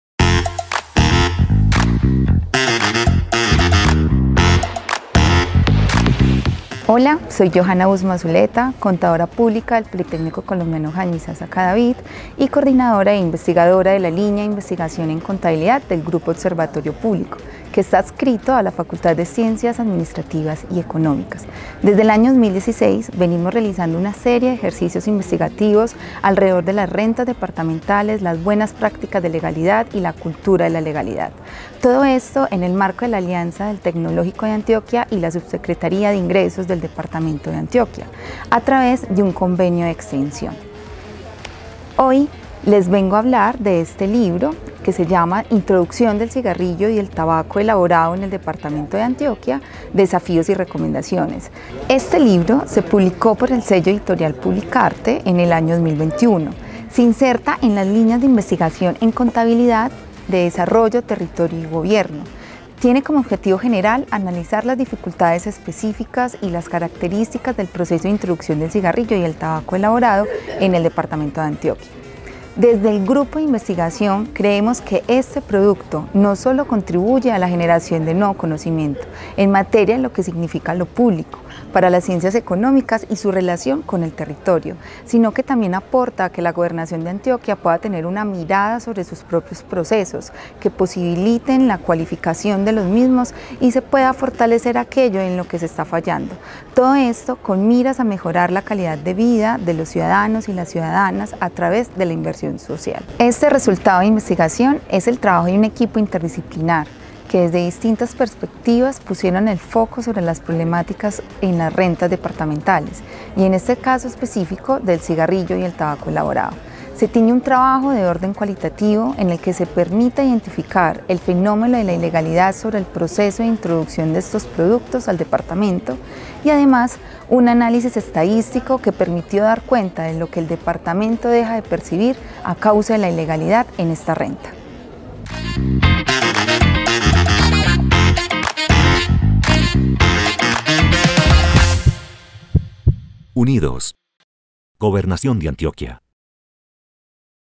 Sello Editorial TdeA - Grupo Observatorio Público
Jhojana nos cuenta el ejercicio que se lleva alrededor de las rentas departamentales, buenas practicas de legalidad. También nos cuenta sobre el libro Introducción del cigarrillo y del tabaco elaborado en el departamento de Antioquia, publicado por el sello editorial del TdeA en el año 2021.
Ven al TdeA.